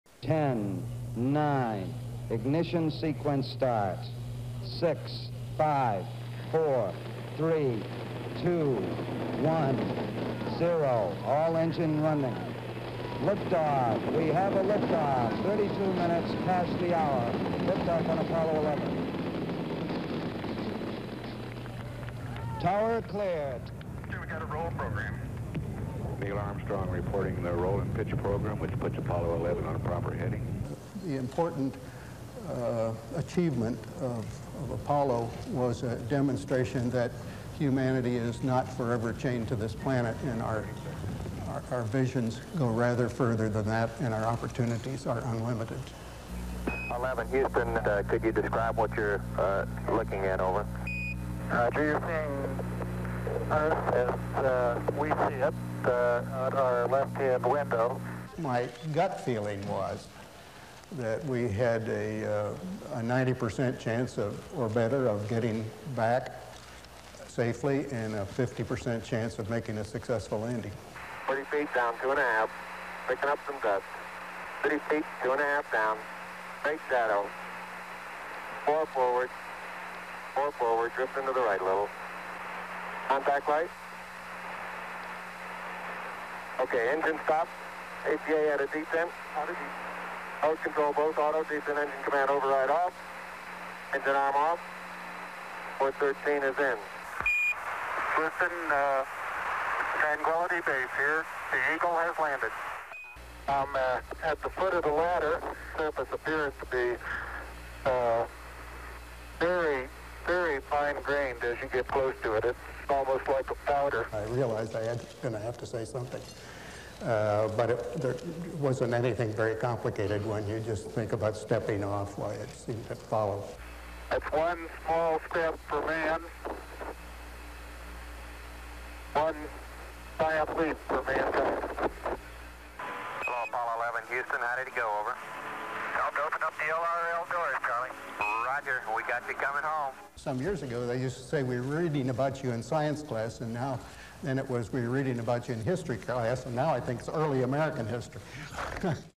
Neil Armstrong's Moon Memories
Apollo 11 commander discusses his lunar achievement (July 20, 1999).